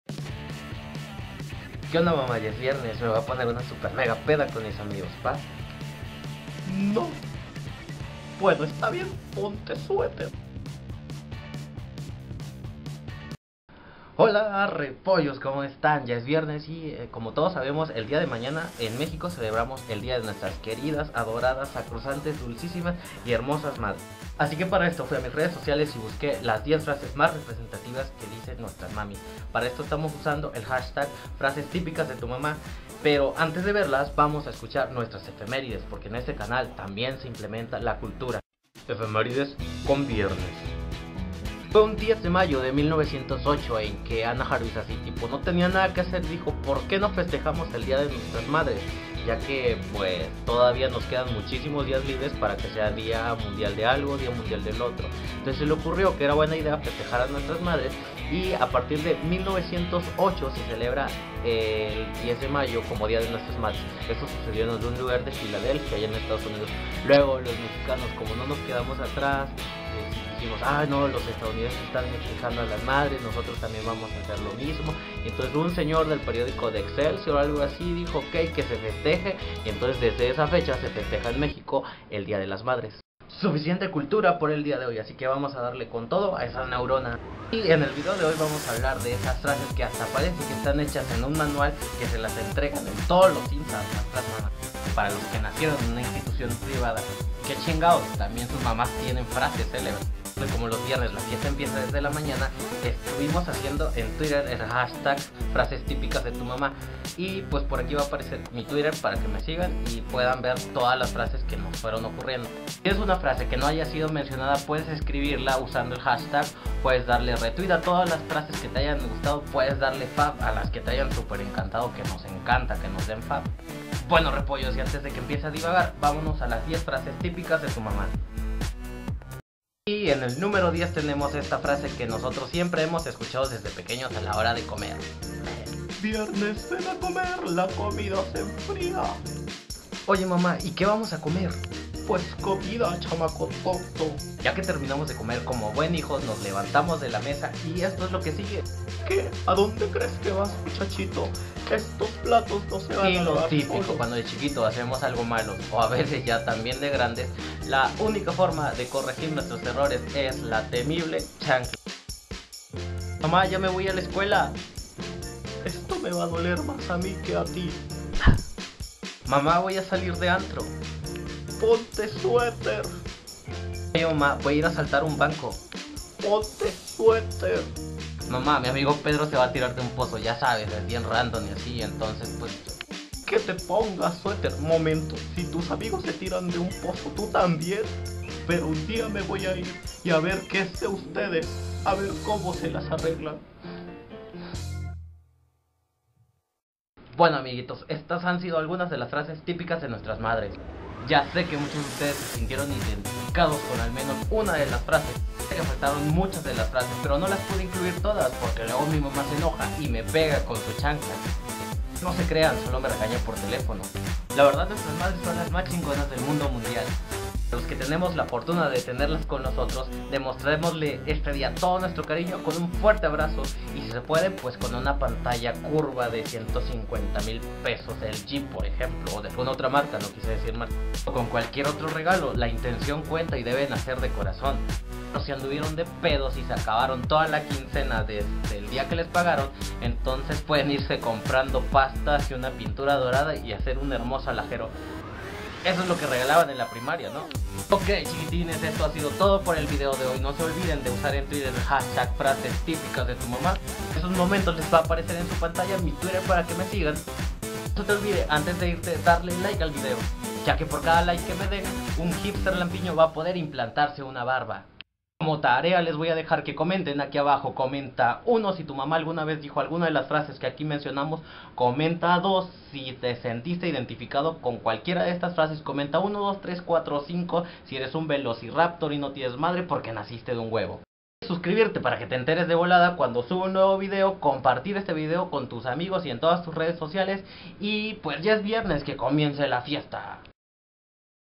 Frases Típicas de Mamá
Las frases que comunmente  usan nuestras  madres, a  poco no?